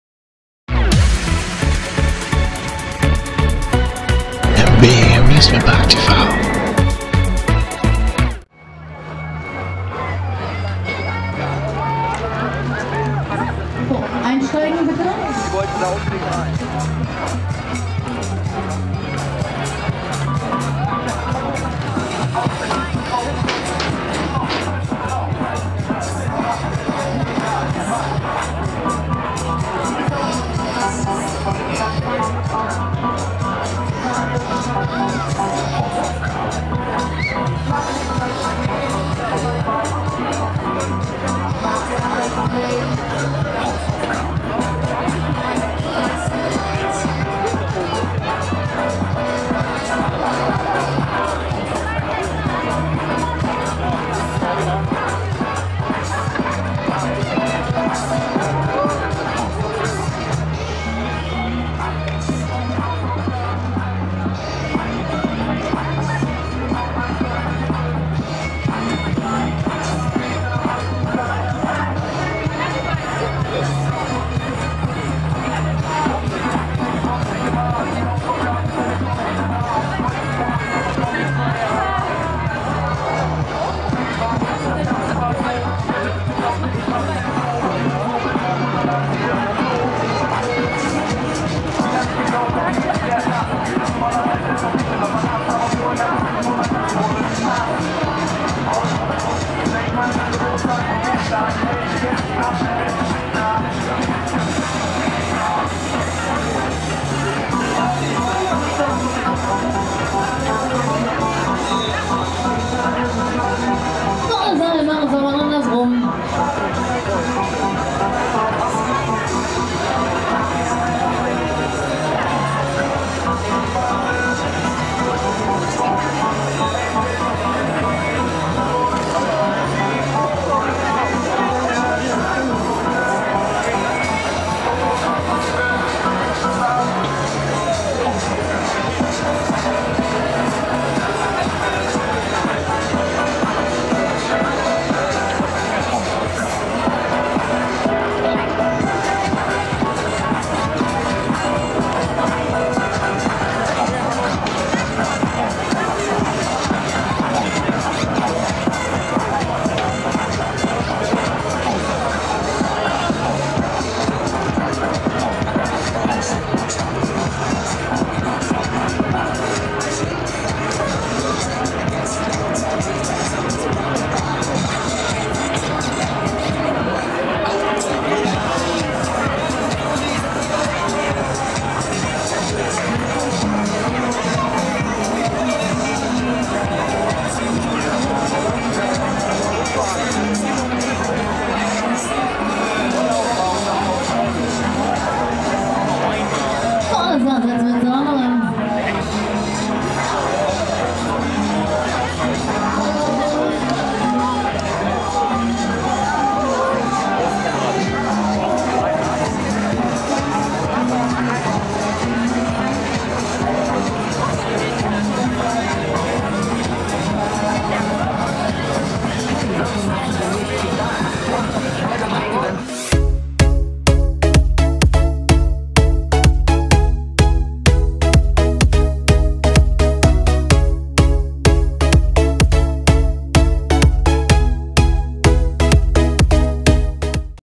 Mach 1/ Fehlauer - Schützenfest Emden 2019
Moin!
In diesem seht ihr den Mach 1, der Schaustellerfamilie Fehlauer aus Börnhöved. Es handelt sich hierbei um einen KMG Speed, der Niederländischen Karussellfirma KMG. Die Anlage, mit dem Baujahr 2010, reiste in ihren ersten Jahren durch Großbritannien unter dem dortig Schausteller Nathan Hart. Seit 2019 wird das Fahrgeschäft von der Familie Fehlauer betrieben. Im Video seht ihr ein Offride der Fahrt auf dem Schützenfest in Emden 2019.